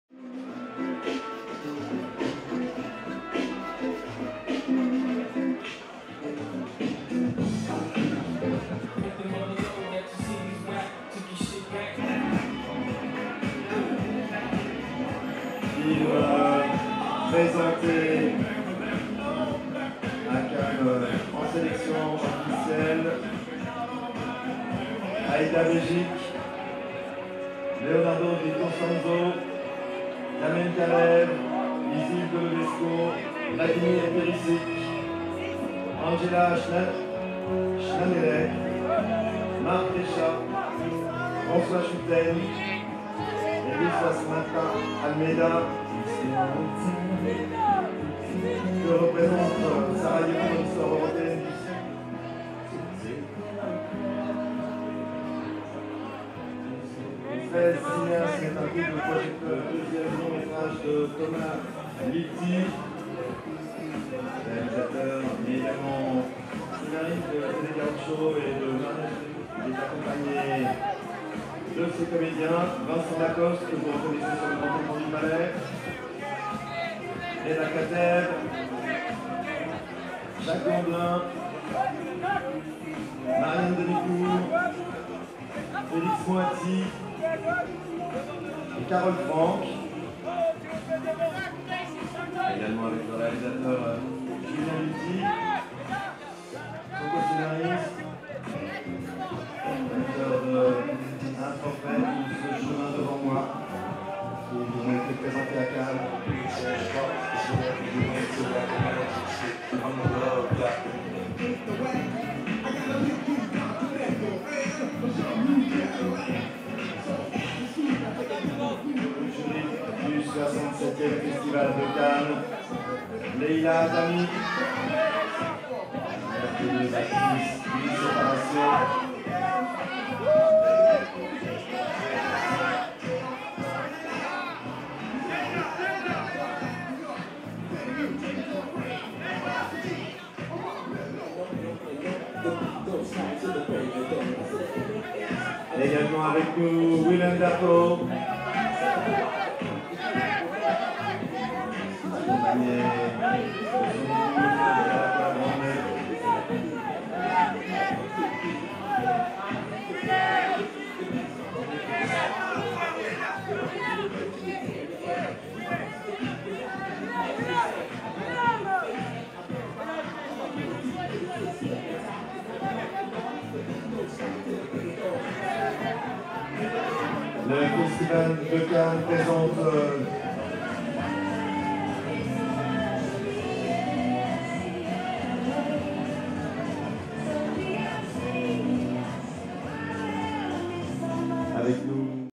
'Jimmy's Hall' premiere during the 67th Annual Cannes Film Festival
Isild Lebesco , Vincent Lacoste, Jacques Gamblin, Willem Dafoe, Giada Colagrande, Reda Kateb, Leila Hata, Agnes B and more attends the 'Jimmy's Hall' premiere during the 67th Annual Cannes Film Festival

May 22, 2014 in Cannes, France